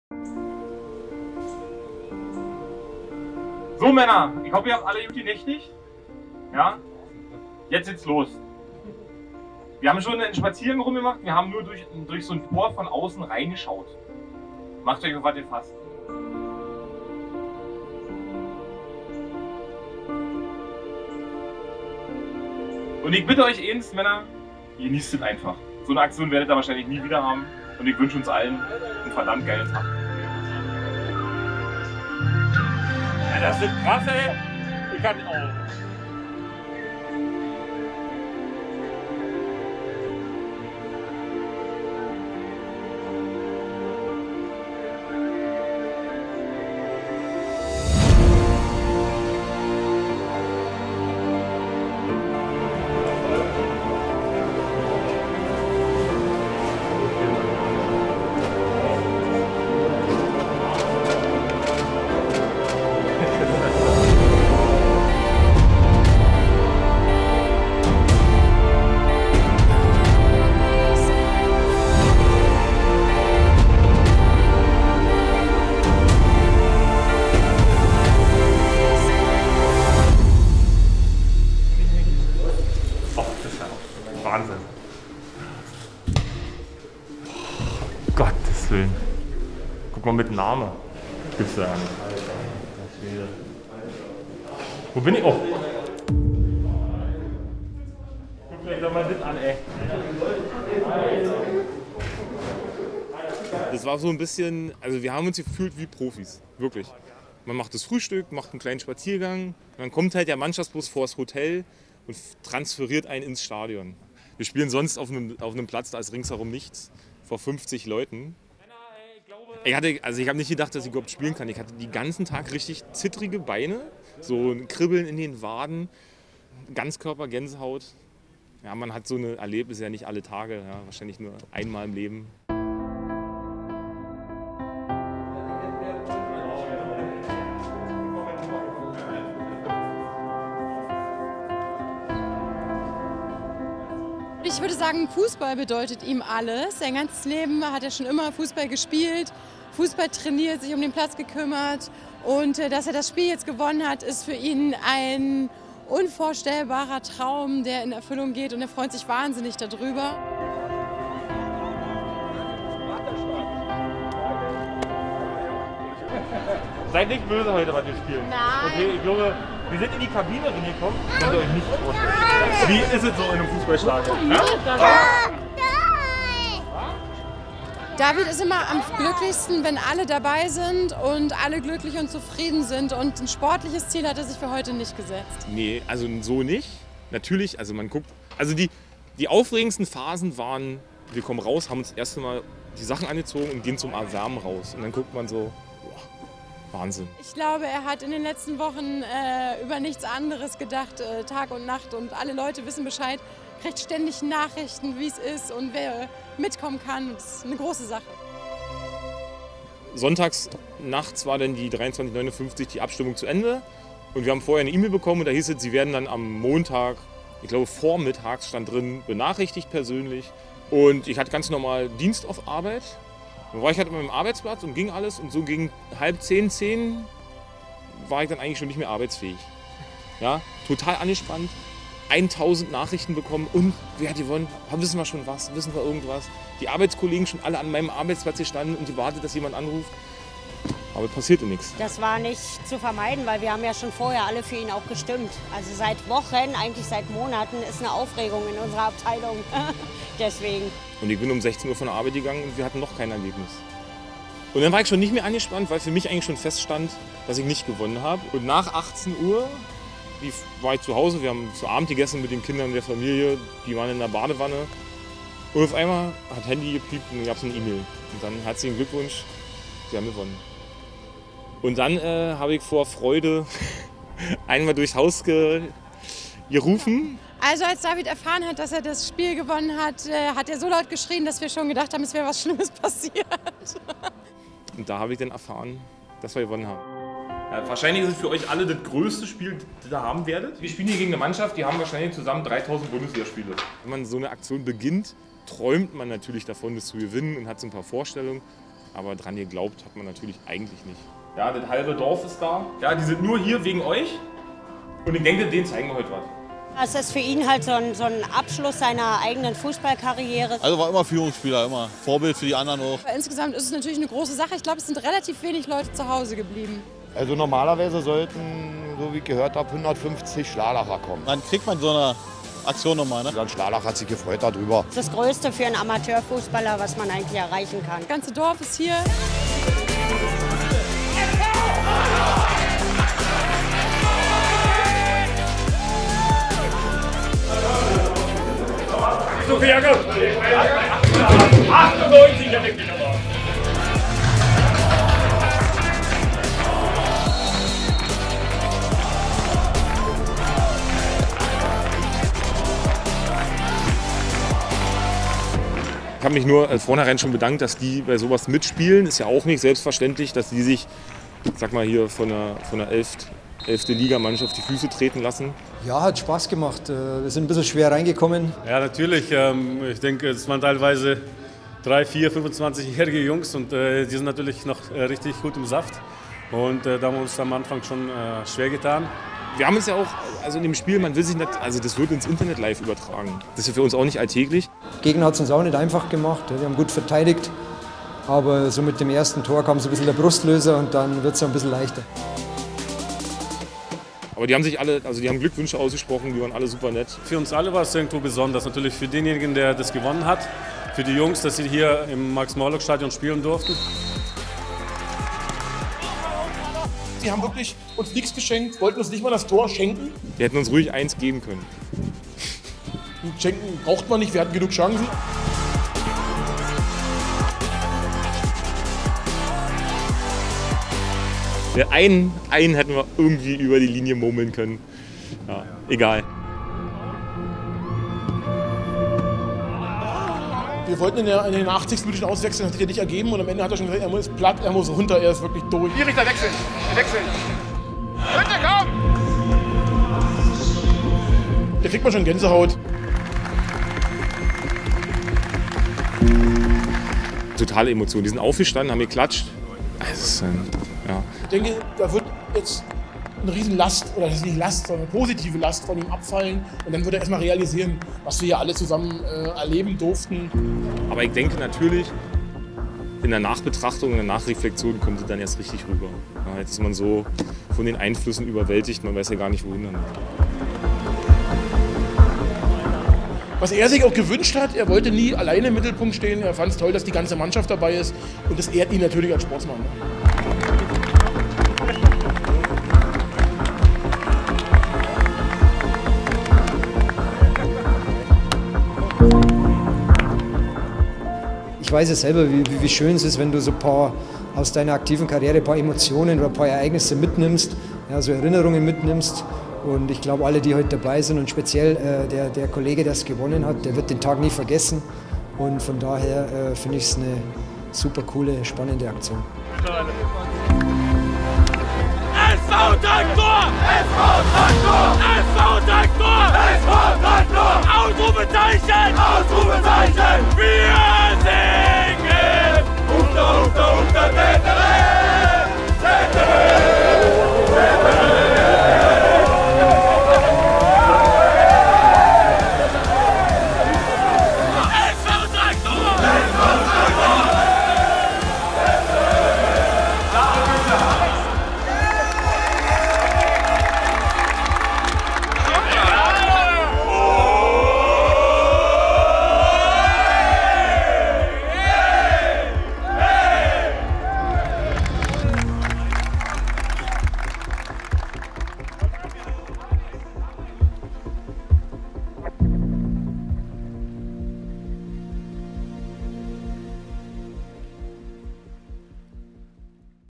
Mein Abschiedsspiel. Mit der NÜRNBERGER.
David "Hütte" Günther hat zusammen mit der NÜRNBERGER Versicherung und dem kicker das Ende seiner aktiven Fußballkarriere gefeiert - als Gewinner der Aktion "Mein Abschiedsspiel". Sowas gab es noch nie!